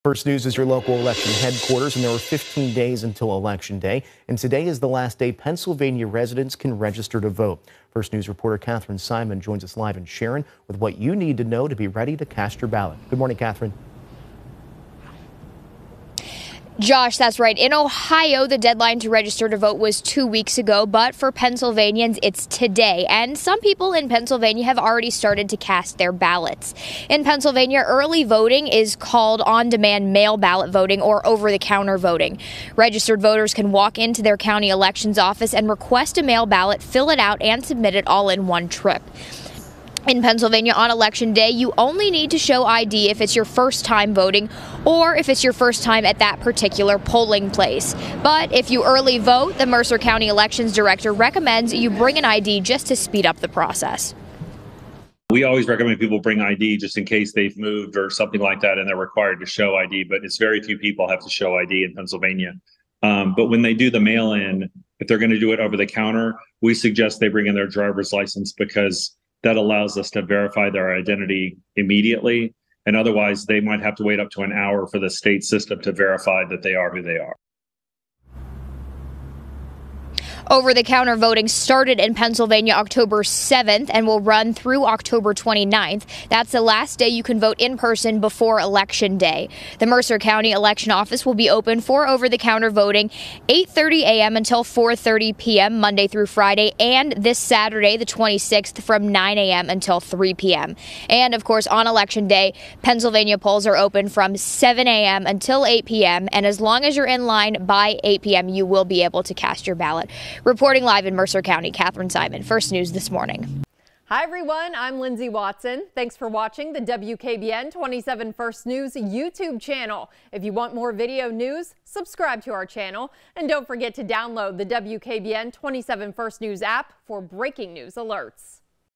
Today is the last day to register to vote in Pa.
How to register to vote in the November election in Pennsylvania